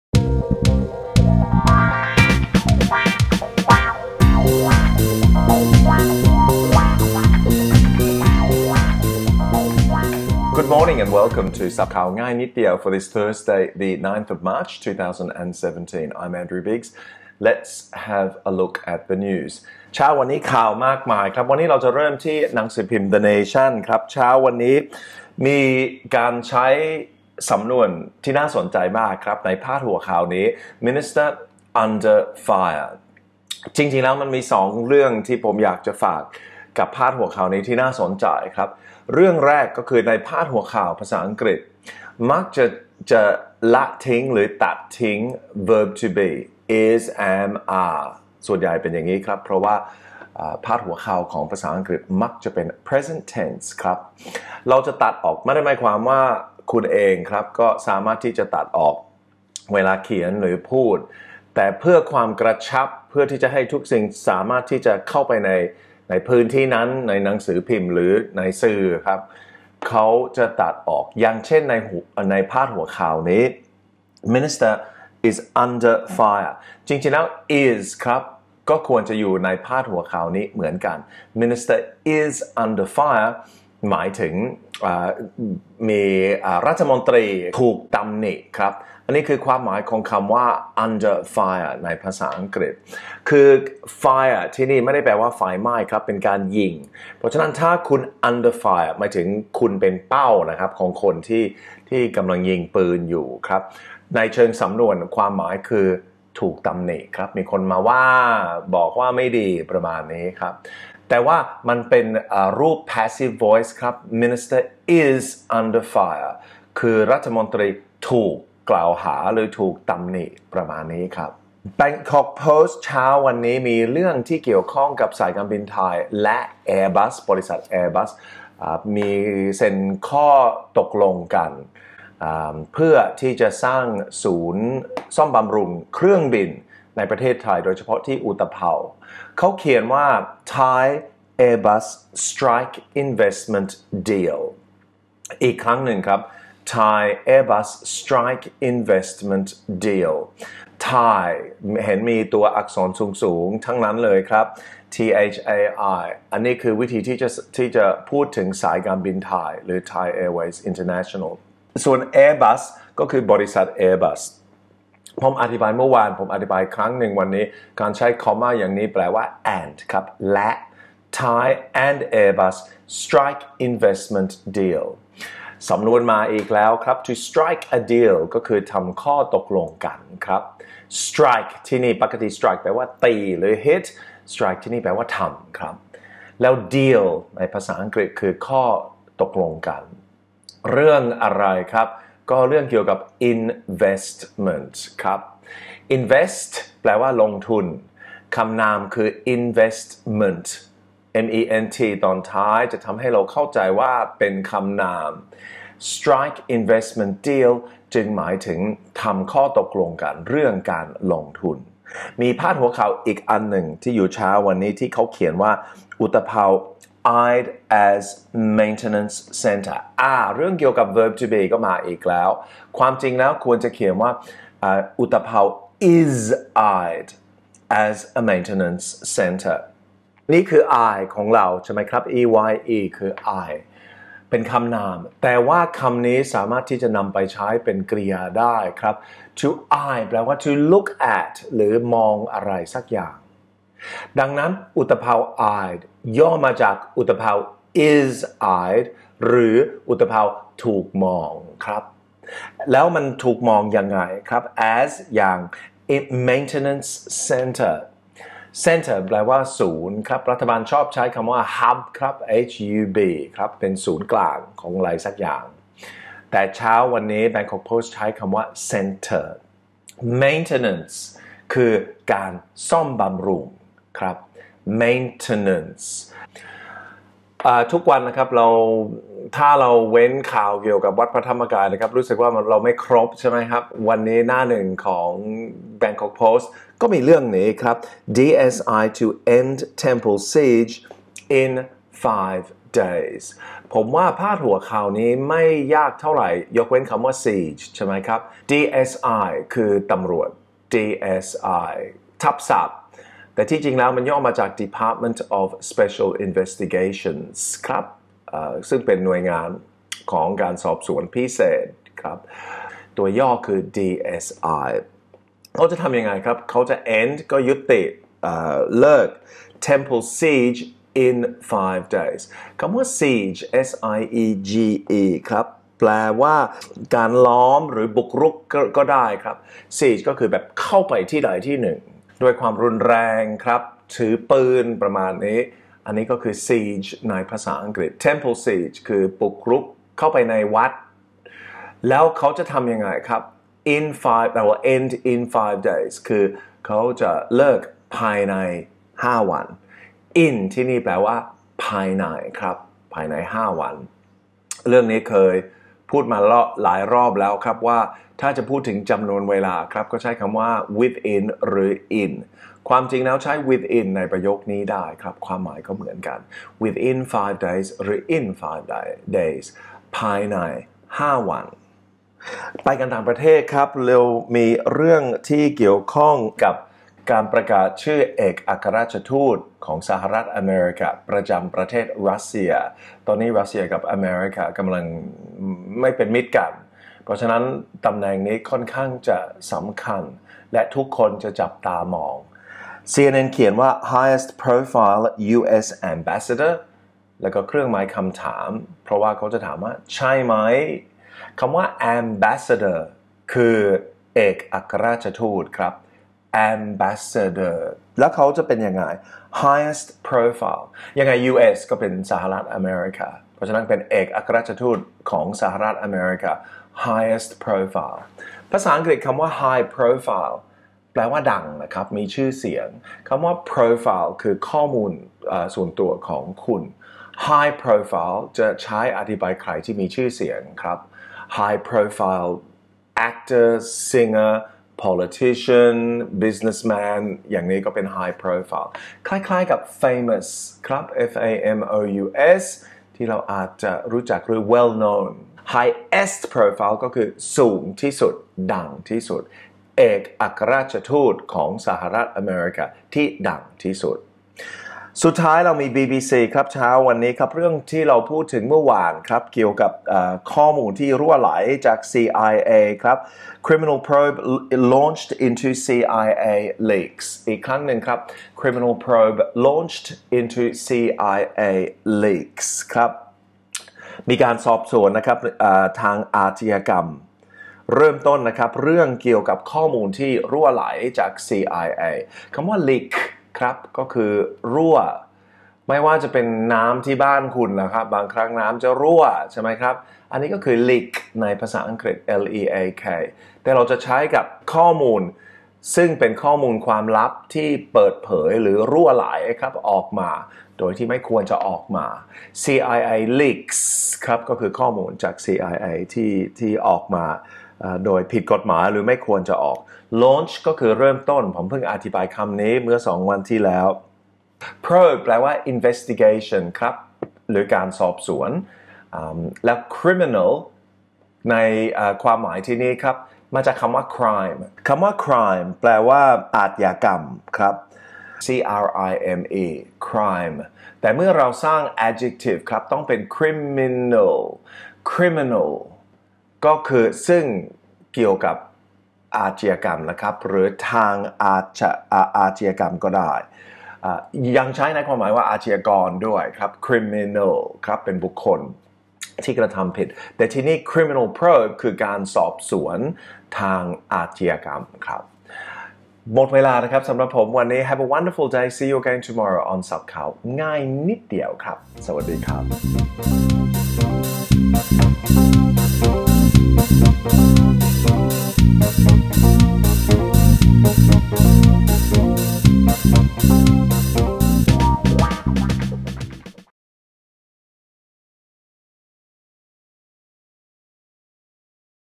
ศัพท์ข่าวง่ายนิดเดียว 09 03 17
ศัพท์ข่าวง่ายนิดเดียว วันที่ 9 มีนาคม พศ. 2560
เตรียมพร้อมรับความรู้ด้วยคำศัพท์จากข่าวในชีวิตประจำวันทุกเช้า จันทร์ถึงศุกร์ กับแอนดรูว์ บิ๊กส์